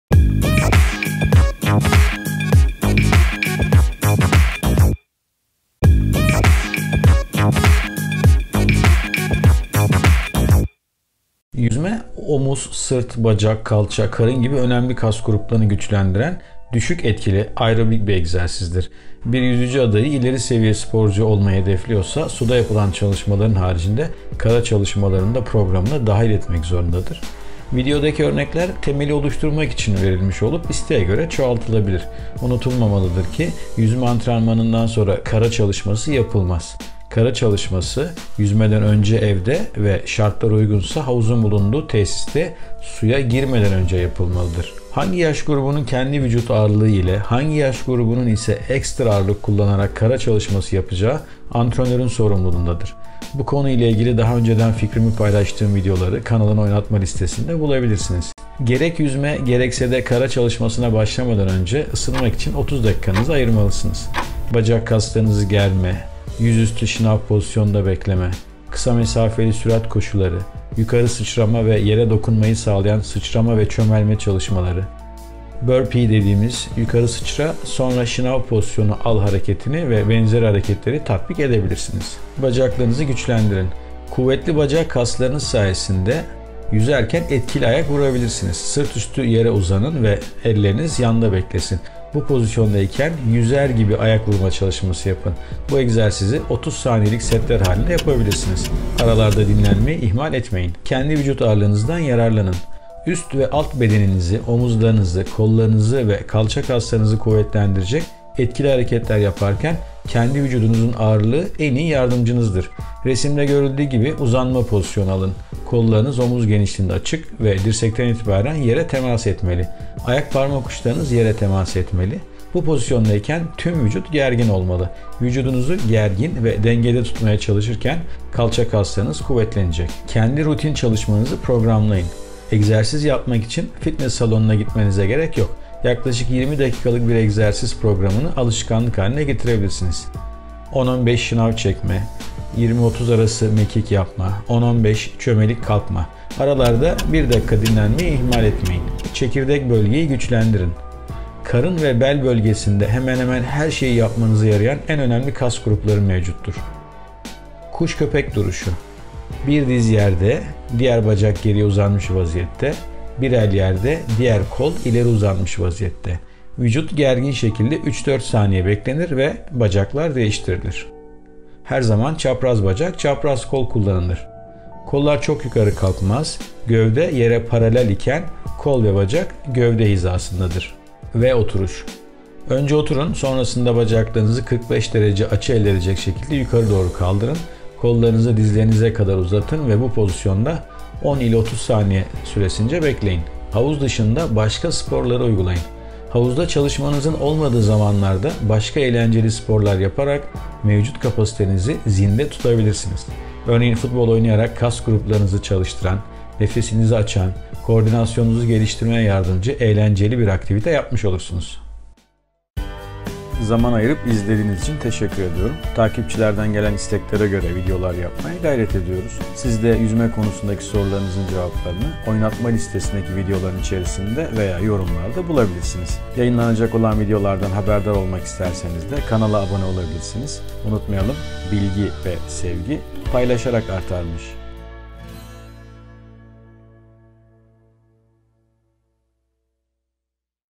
Yüzücüler için HAVUZ DIŞI Antrenman Tavsiyesi
Yüzme; omuz, sırt, bacak, kalça, karın gibi önemli kas gruplarını güçlendiren düşük etkili aerobik bir egzersizdir. Bir yüzücü adayı ileri seviye sporcu olmayı hedefliyorsa; suda yapılan çalışmaların haricinde kara çalışmalarını da programına dahil etmek zorundadır.

Videodaki örnekler temeli oluşturmak için verilmiş olup, isteğe göre çoğaltılabilir. Unutulmamalıdır ki; yüzme antrenmanından sonra kara çalışması yapılmaz! Kara çalışması yüzmeden önce evde veya şartlar uygunsa havuzun bulunduğu tesiste suya girmeden önce yapılmalıdır.

Hangi yaş grubunun kendi vücut ağırlı ile, hangi yaş grubunun ise ekstra ağırlık kullanarak kara çalışması yapacağı antrenörün sorumluluğundadır. Bu konu ile ilgili daha önceden fikrimi paylaştığım videoları kanalın oynatma listesinde bulabilirsiniz.